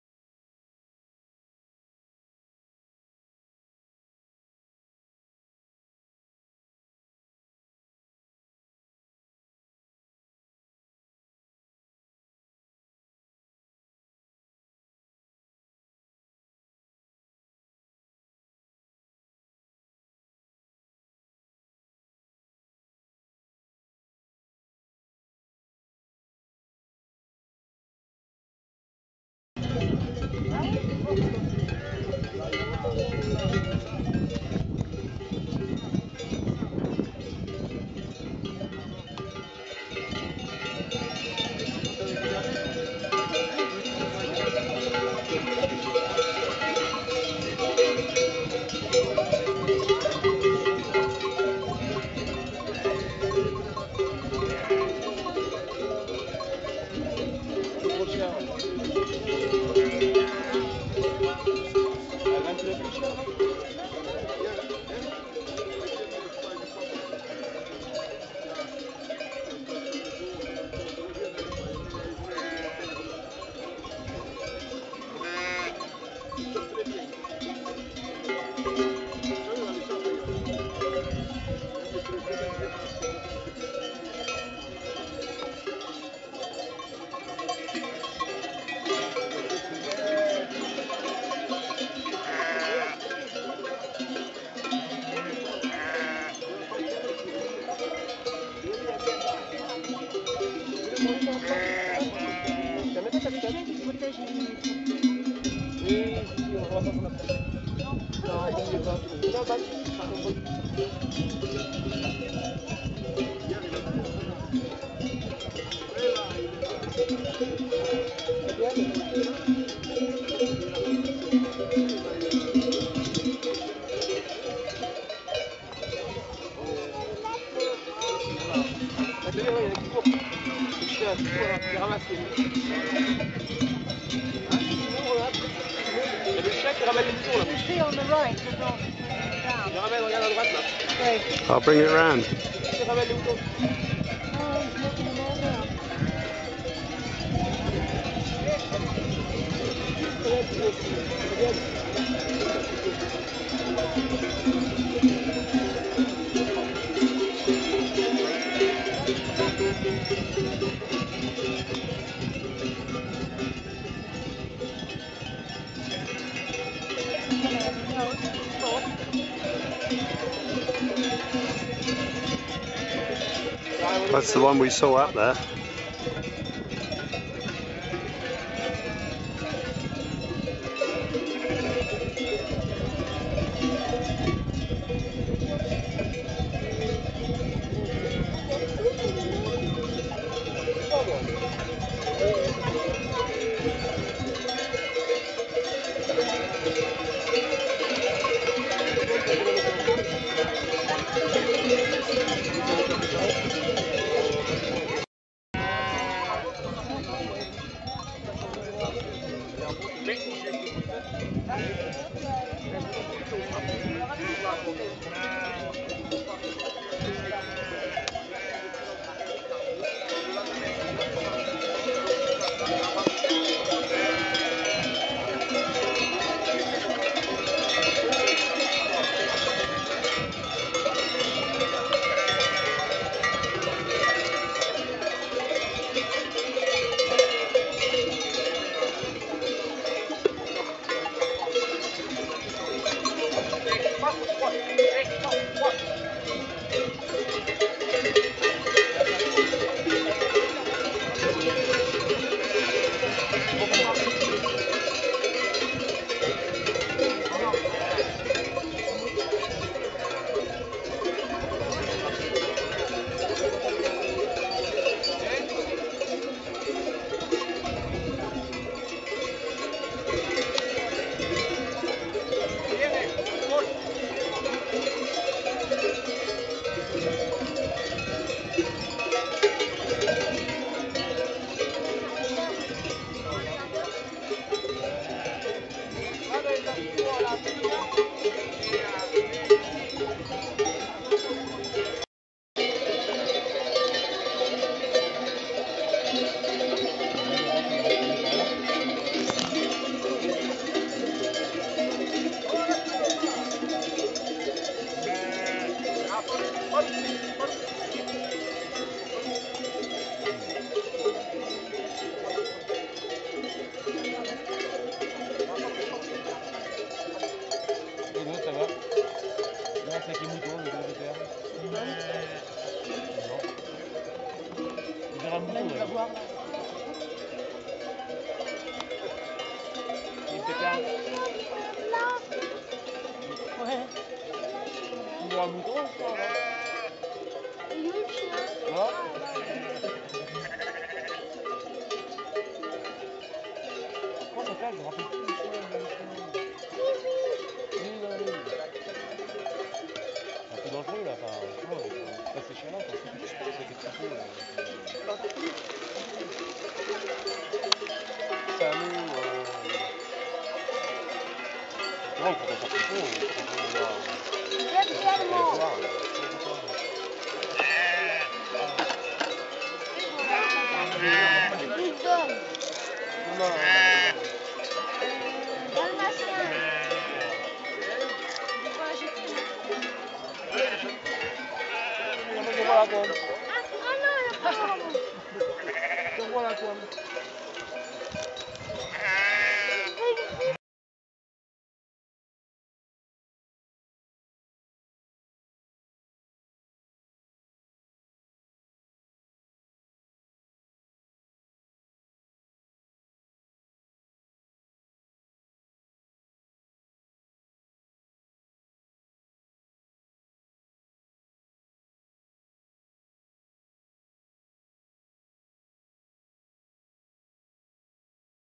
Sheep Herding in the Pyrenees
Pyrenean mountain dog's job is to guard the flock, not to do the running around! Video by author Graham Bishop